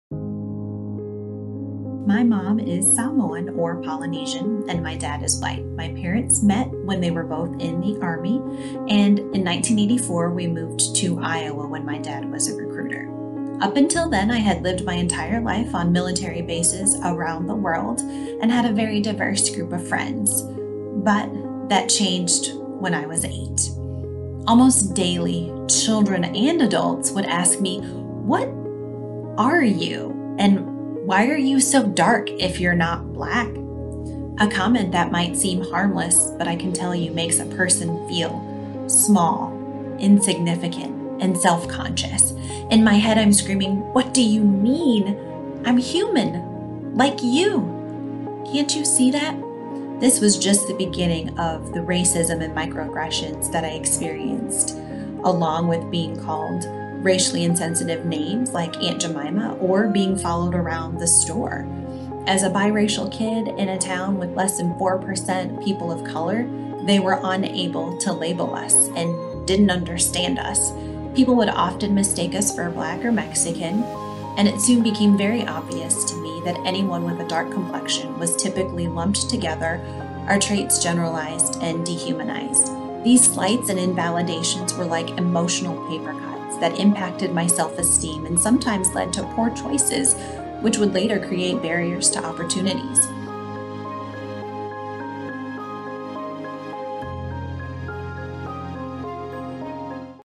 Angelica’s Story
Angelica Vannatta of United Way of East Central Iowa talks about how equity issues have affected her life. Register for the 21-Day Equity Challenge at iowaequitychallenge.org.